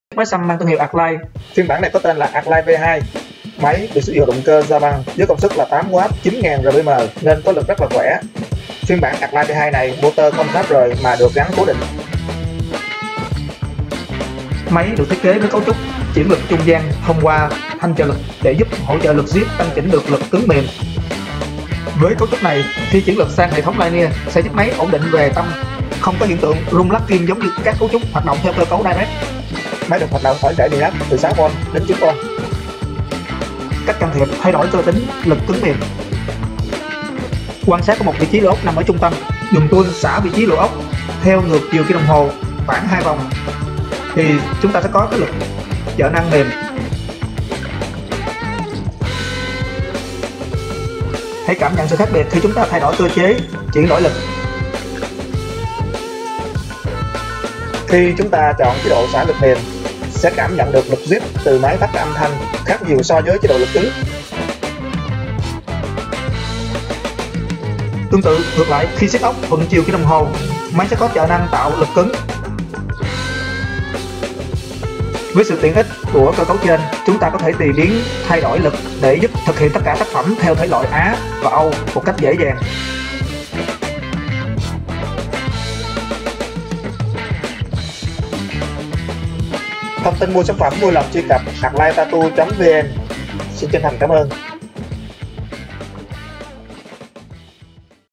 Hướng Dẫn Sử Dụng Máy Xăm Artfly V2 - Tattoo Machine
Thông số kỹ thuât của máy xăm ARTFLY MARK 2 ROTARY
 ( JAPAN MOTOR  )
THÔNG SỐ KỸ THUẬT
Vật liệu khung body : Hợp kim Aluminum 7075
Motor : JAPAN Motor 9000RPM  xuất sứ tại NHẬT BẢN
Mức volt sử dụng : 5v - 9v
TÍNH NĂNG :
Sử dụng kim đầu đạn ( Cartridges )
Stroke cố định 3.8mm
Có thể hỗ trợ can thiệp tùy chỉnh độ zip về lực cứng hoặc mềm để đáp ứng nhu câu đa dạng của tất cả thể loại tác phẩm
Sử dụng kim nét ( liner ) không giới hạn cho tất cả size kim
Sử dụng kim bóng và vào màu sắc ( shader - color ) không giới hạn cho tất cả size kim
Body gọn nhẹ dễ dàn thao thác khi thực hiện tác phẩm
Kích thước đường kính tay nắm theo thông số ch 30mm
Được xử lý màu sắc bằng công nghệ Anode theo tiêu chuẩn ISO thế giới
Có 3 màu sắc : Cam - Đen - Xám